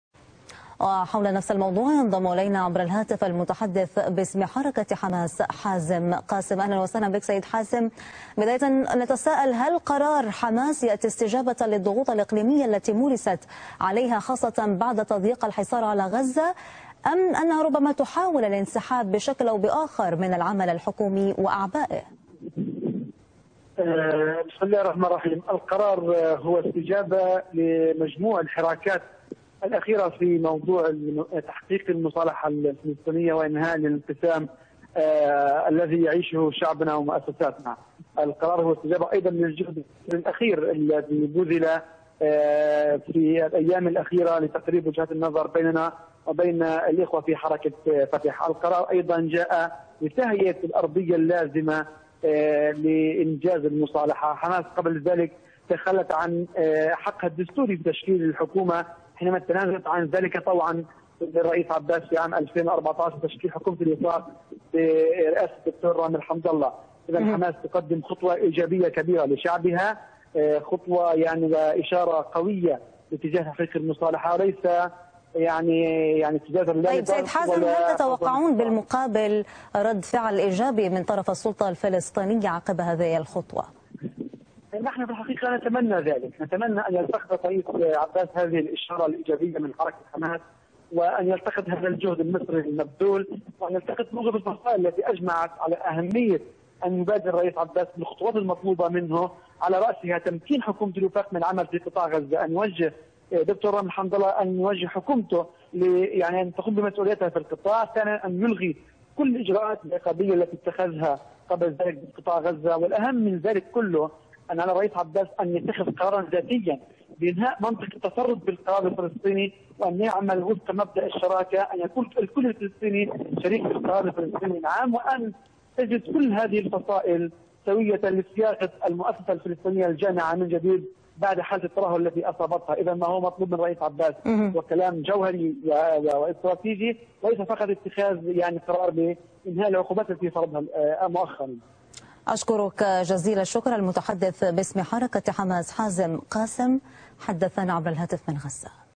التلفزيون العربي | المتحدث بإسم حماس: حل اللجنة الإدارية في غزة خطوة نحو المصالحة
قال المتحدث بإسم حركة المقاومة الإسلامية الفلسطينية "حماس" حازم قاسم إن قرار حل اللجنة الإدارية في قطاع غزة جاء كاستجابة للتحركات الأخيرة التي تهدف لتحقيق المصالحة الفلسطينية وإنهاء الانقسام الذي يعيشه الشعب الفلسطيني، مؤكدا أن هذه الخطوة تأتي في ظل الجهود الإقليمية التي تم بذلها مؤخرا لتقريب وجهات النظر بين الأطراف الفلسطينية.